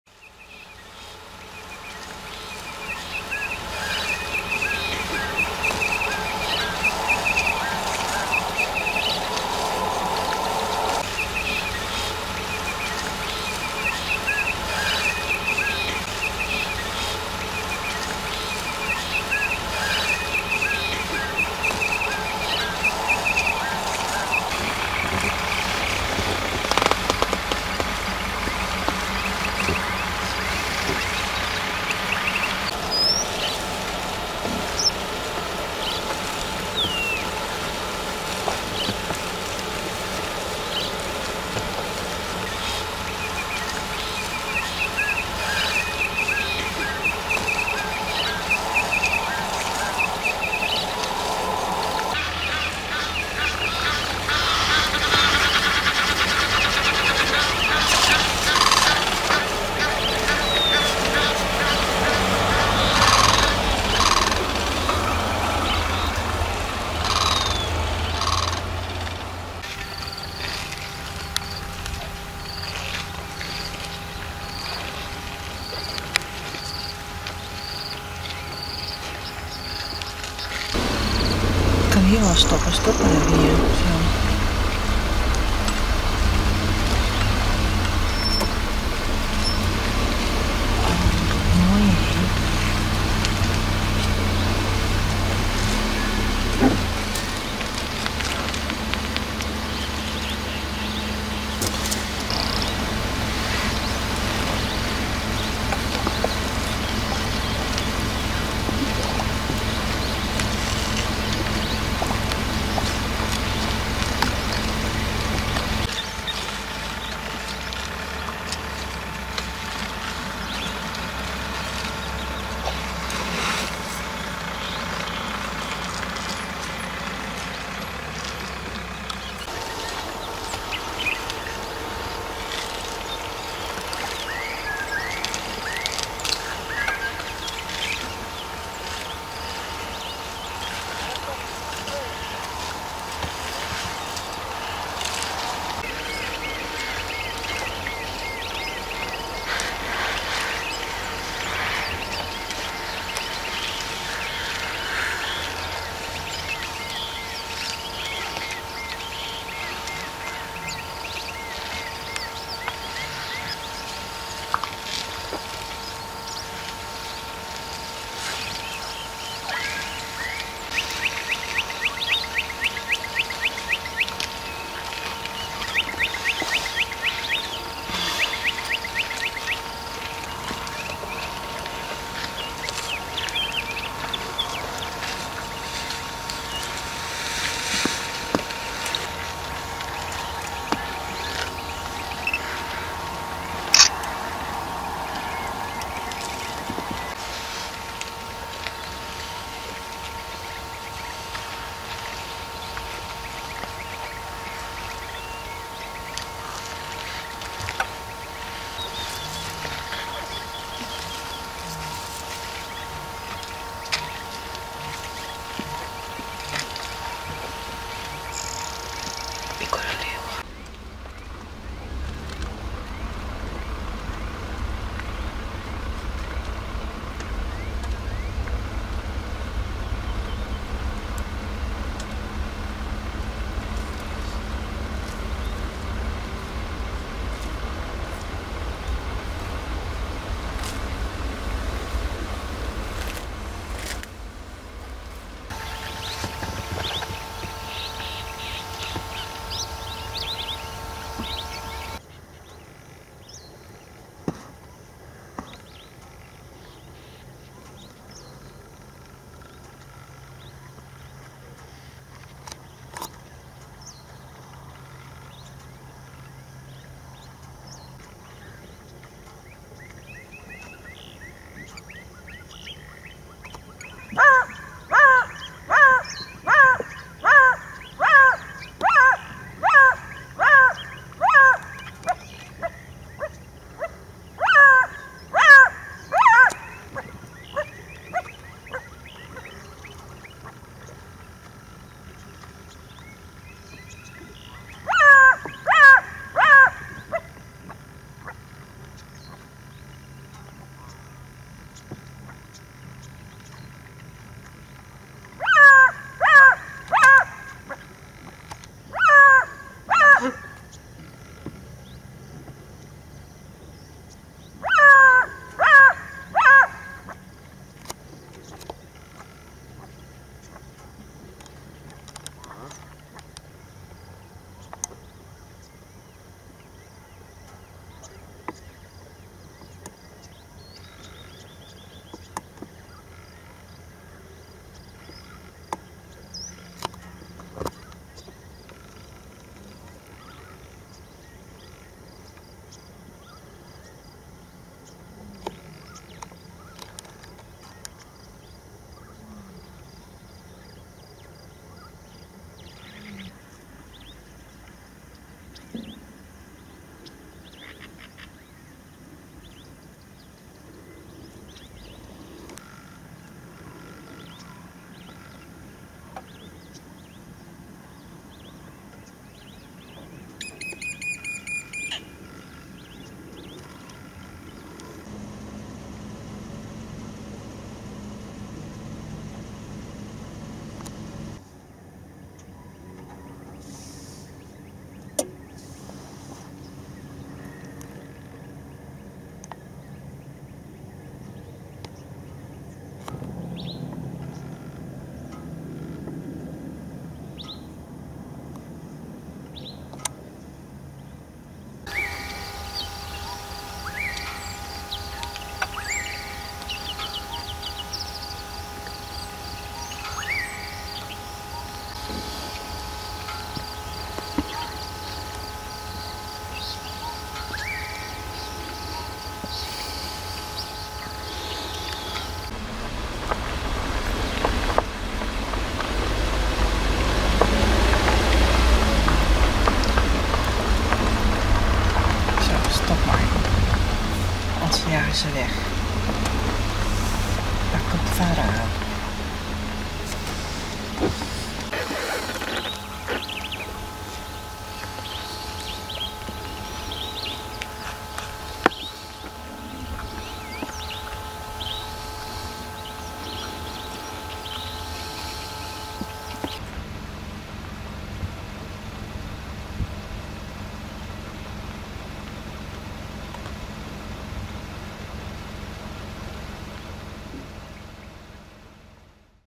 SOUTH AFRICA hyena, secretary bird and more, Addo Elephant national Park (28 February 2014)
In February/March 2014 we visited seven national parks. Join us on our game drives day by day.
Our second stop was Addo. In the morning of the second day we spotted elephant, buffalo, hyena, warthog, jackal and secretary bird.
(original sounds).
Addo Elephant National Park is a diverse wildlife conservation park in South Africa and is one of the country's 20 national parks. It currently ranks third in size after Kruger National Park and the Kgalagadi Transfrontier Park.
The flora within the park is quite varied, and like all plant life, is a central factor to the ecological system in place. Several species of rare and endemic plants, particularly succulent shrubs are native to the South African region within the park. Many species are under environmental pressure, however, and are facing possible extinction.
More than 600 elephants, 400 Cape buffaloes, over 48 endangered black rhinos as well as a variety of antelope species. Lion and spotted hyena have also been re-introduced to the area. The largest remaining population of the flightless dung beetle is located within the park.